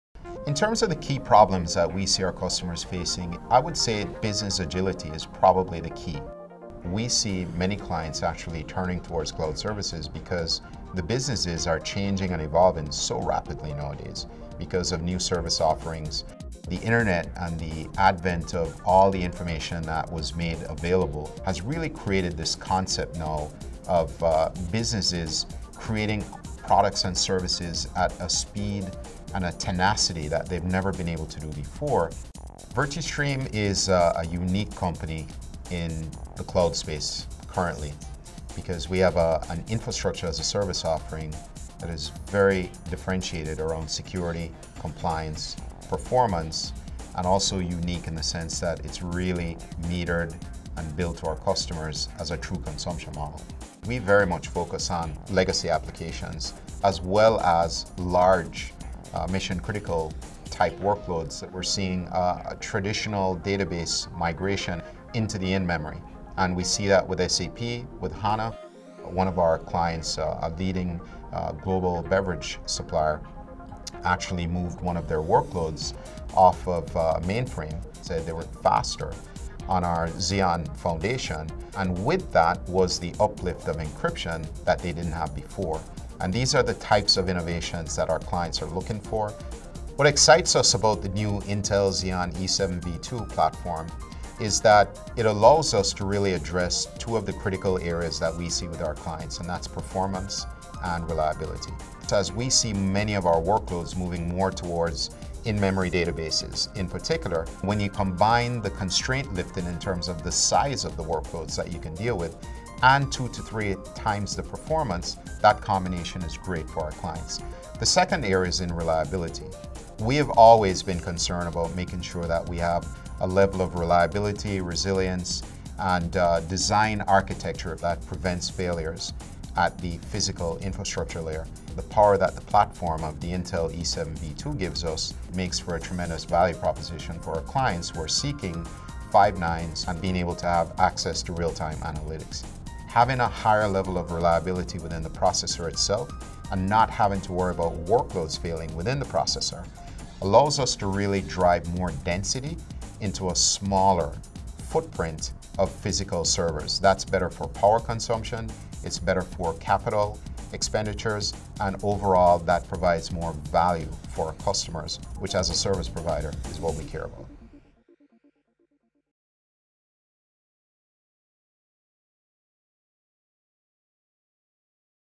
Reliability & Real Time Workloads With Virtustream & The Intel Xeon Processor E7 V2
How is Virtustream able to deliver Infrastructure as a Service (IaaS) for mission critical and real time workloads? Find out as Kevin Reid, Co-CEO outlines their use of the new Intel Xeon Processor E7 v2.